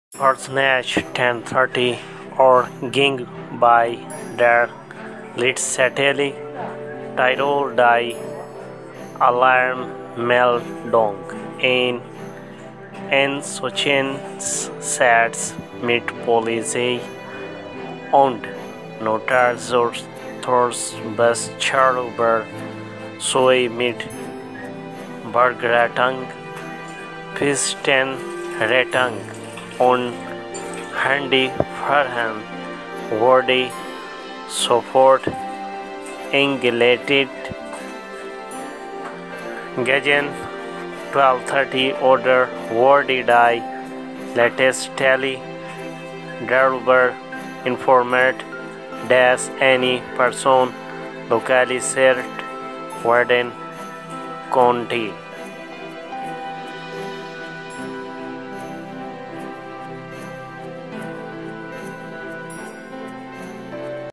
Frau kam bei Lawinenabgang in Tirol ums Leben
Kurz nach 10.30 Uhr ging bei der Leitstelle Tirol die Alarmmeldung ein. Ein Sucheinsatz mit Polizei- und Notarzthubschrauber sowie mit Bergrettung, Pistenrettung und Hundeführern wurde sofort eingeleitet.

 Gegen 12.30 Uhr wurde die Leitstelle darüber informiert, dass eine Person lokalisiert werden konnte. Auf Nachfrage der „Krone“ bestätigte die Leitstelle zunächst, dass die verschüttete Person aus den Schneemassen geborgen werden konnte. Für sie kam jedoch jede Hilfe zu spät.